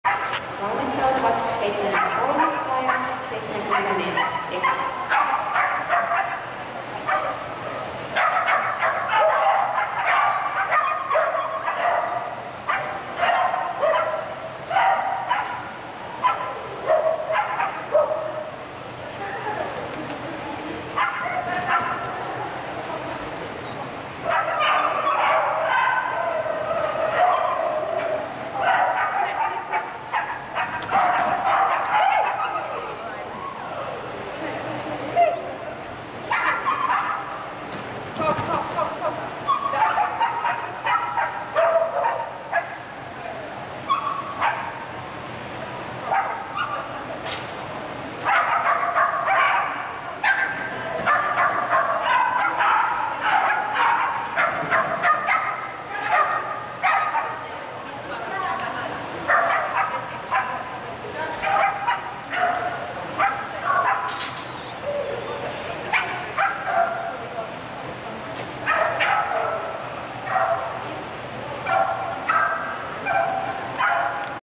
C,M,71 Bessy
rata C , M , Bordercollie , Bessy ( Aisknekht Every Time The Bast ) , rv hylätty , tuomari Anders Virtanen , ATT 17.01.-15 , kuvaus Vekara Agility